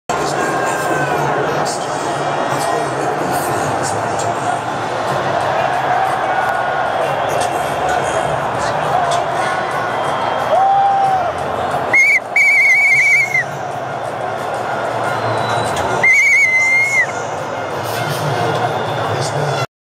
Colombia vs Costa de marfil en Brasilia
El estadio llenandose poco a poco